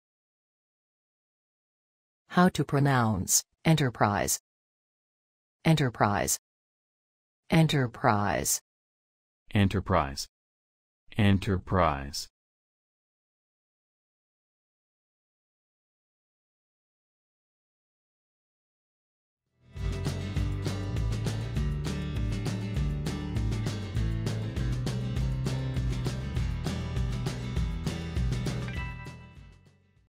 How to say Enterprise
How to pronounce Enterprise in English? Listen with us.
What is the correct pronunciation of the word Enterprise in everyday English? Speak as the Americans.
How to write the word Enterprise? Watch and learn.

Learn how to pronounce Enterprise.